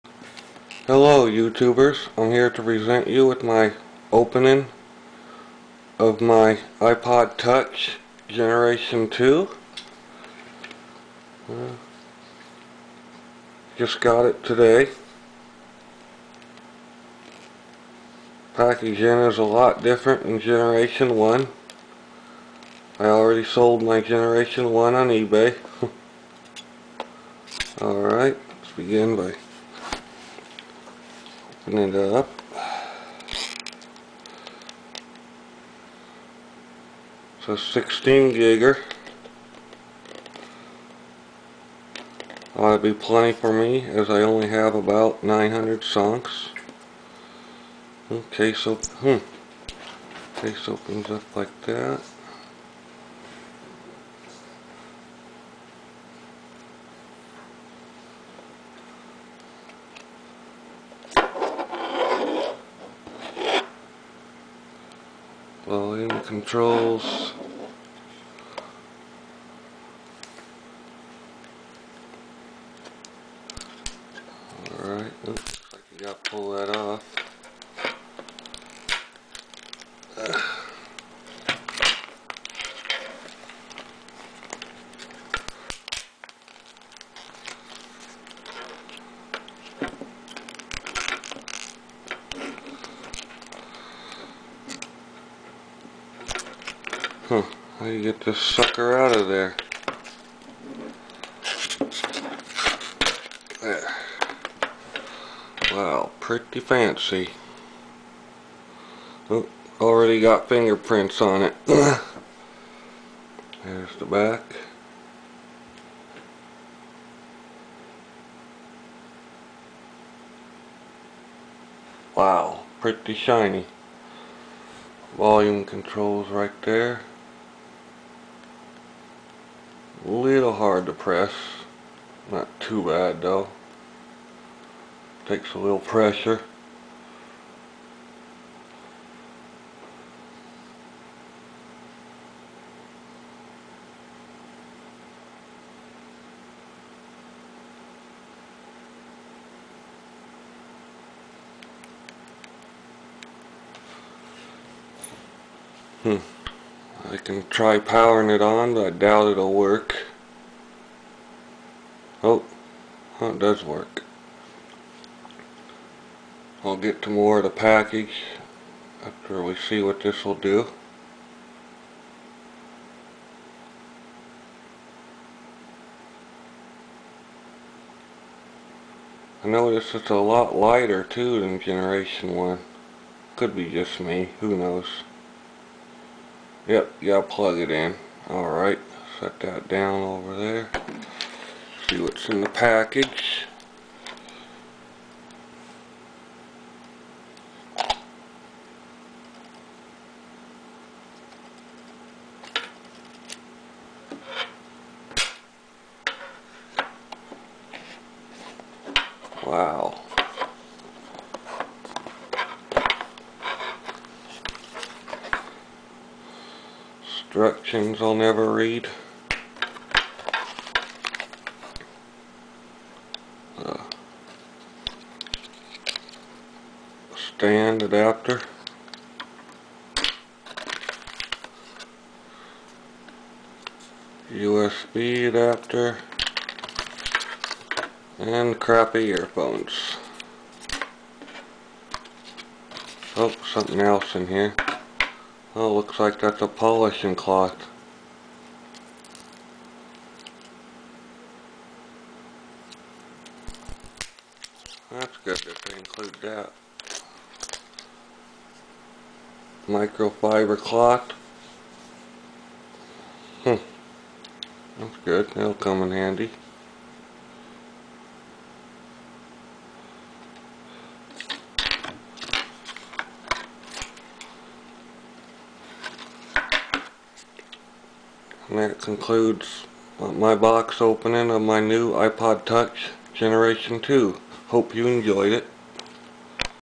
iPod Touch 2nd Generation 2G Unboxing
If you want to help me get a DJI Mavic drone so I can get awesome aerial video of Nebraska you may donate

I got my New iPod Touch Gen2 Today and I am going to present you the unboxing of it.  Enjoy!! Please Rate my video if you feel i deserve it!!